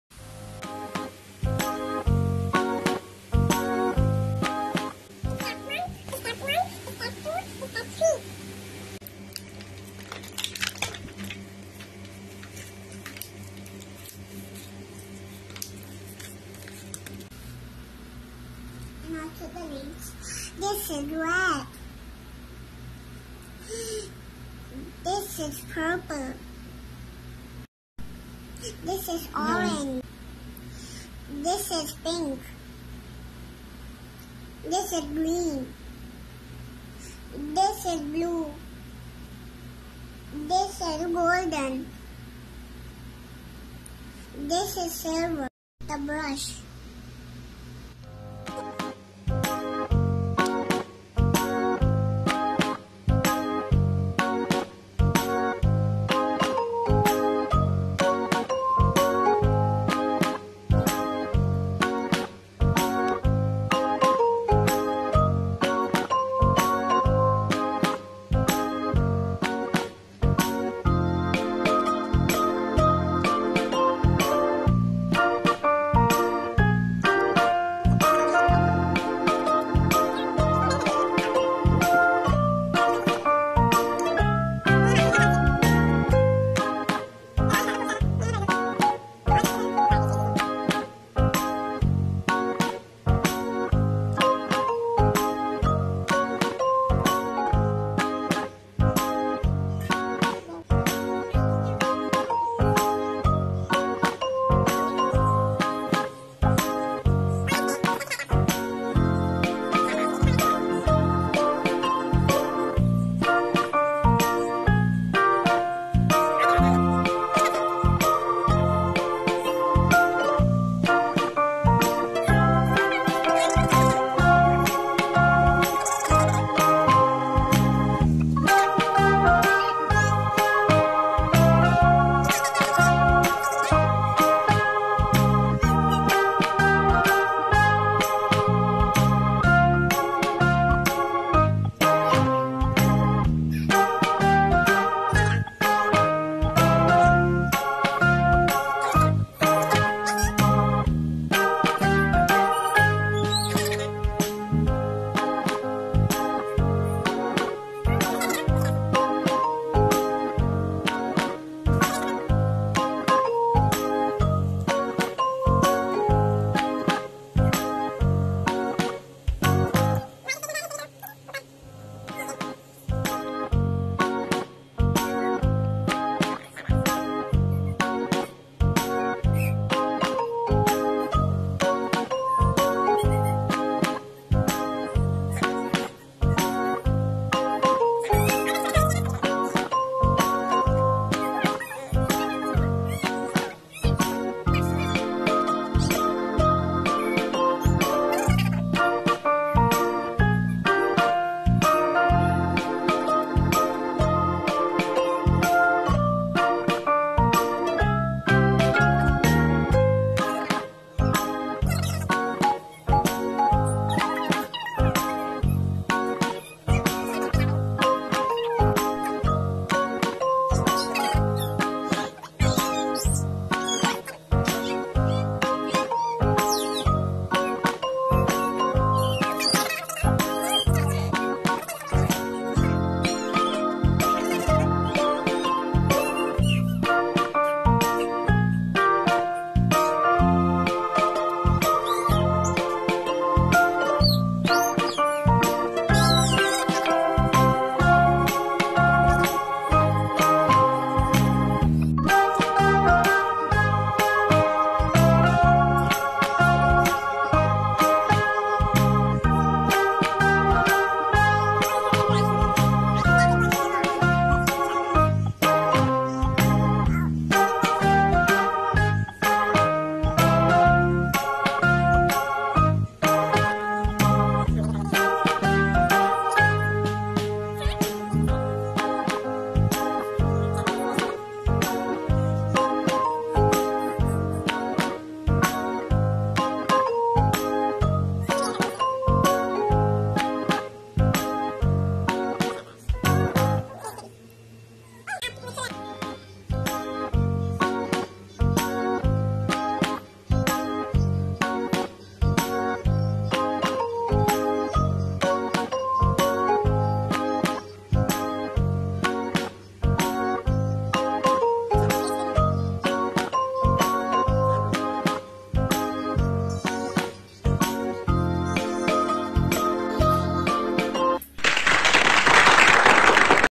Easy painting tutorial for kids..learn colors with The Mom and Fatima
That was a cheap paint set yet very interesting and easy.Fatima loved doing it.kids can easily learn coloring from this video